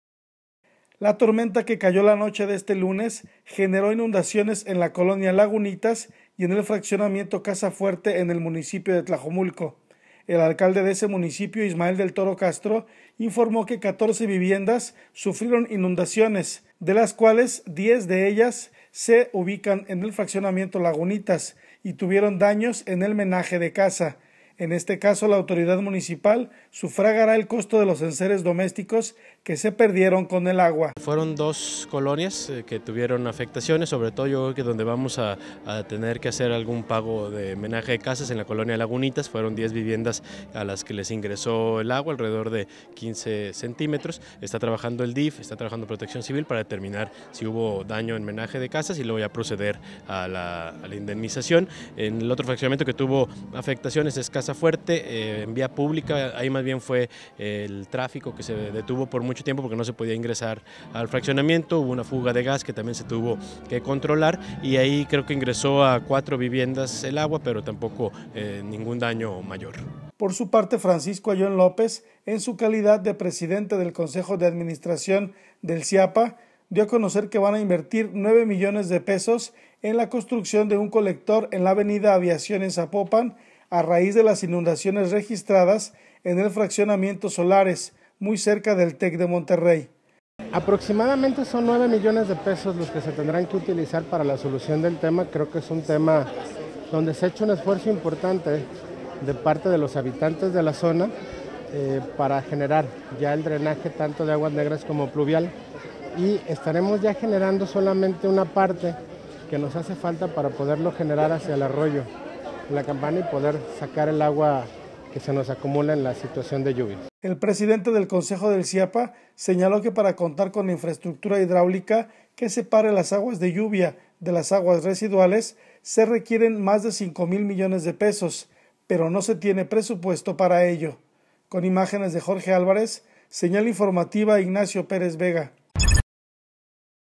Señal Informativa: 20 viviendas en Tlajomulco son afectadas por lluvias
La lluvia azotó la noche del lunes en la zona metropolitana de Guadalajara, inundó 20 viviendas en Tlajomulco, informa el alcalde Ismael del Toro.